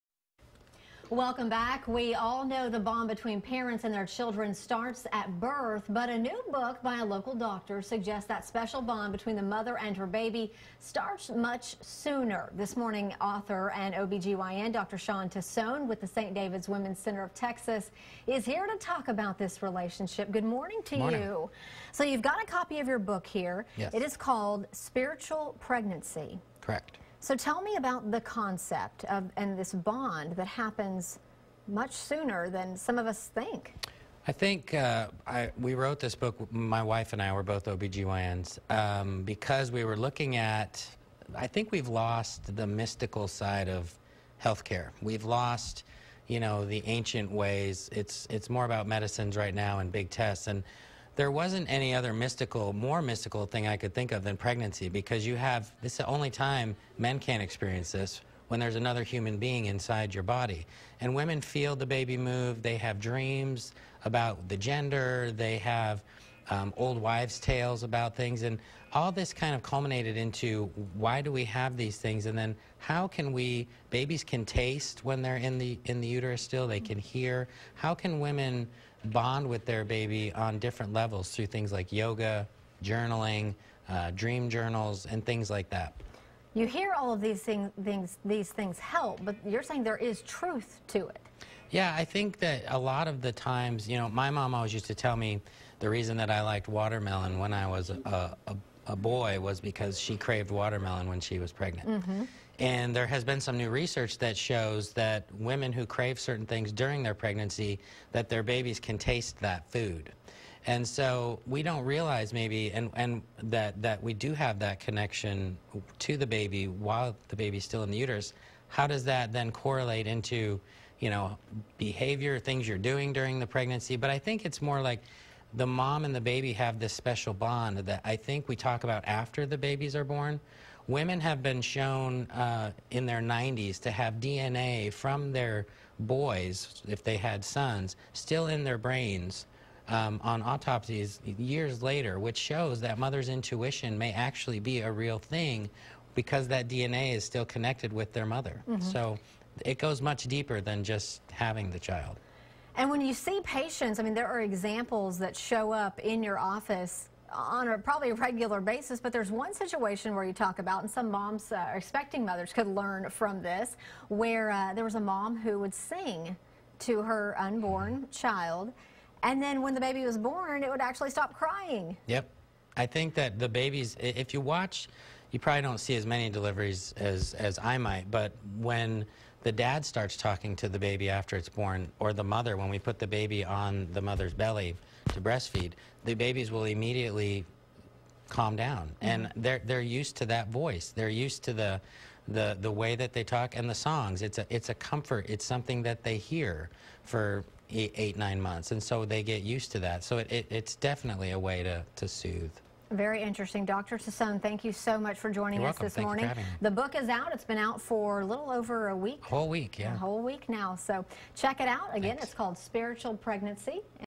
Bonding before birth
A new book by a local doctor suggests that the special bond between a mother and her baby begins before the baby is ever born.